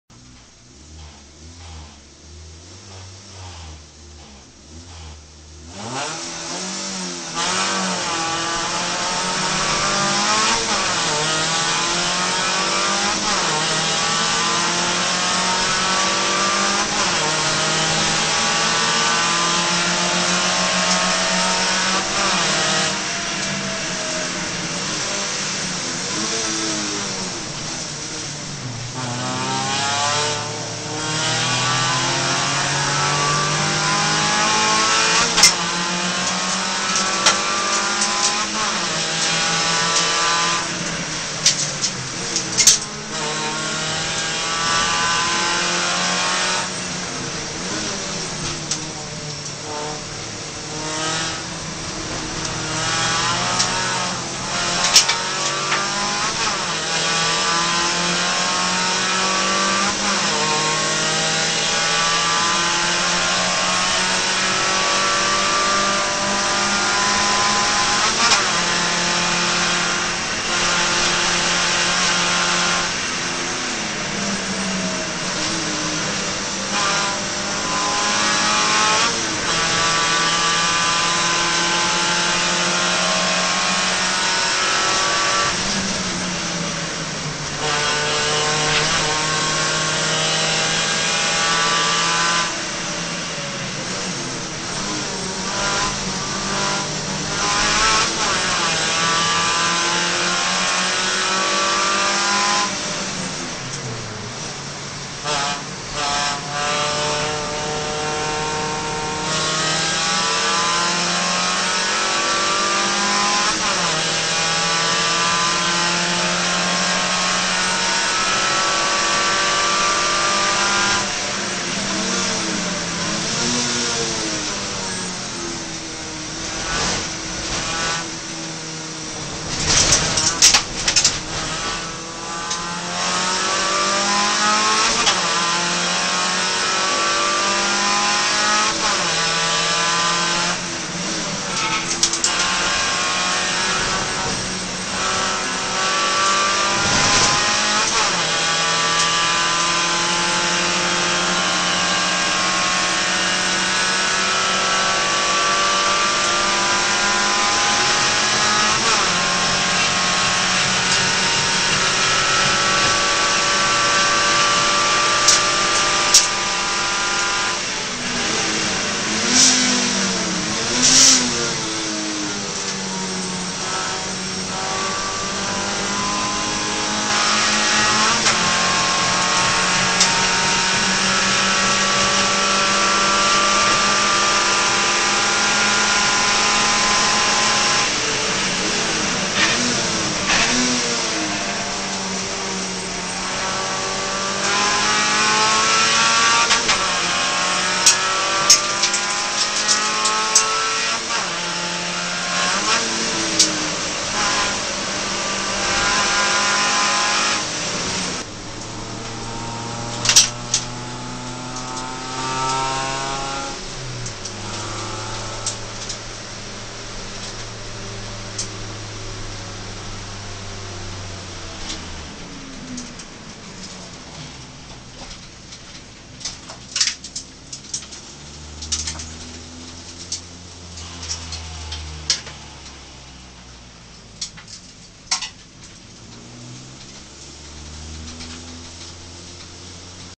Onboard: Hugo Charles
Onboard bij Hugo Charles in de Formido Swift Cup 2011.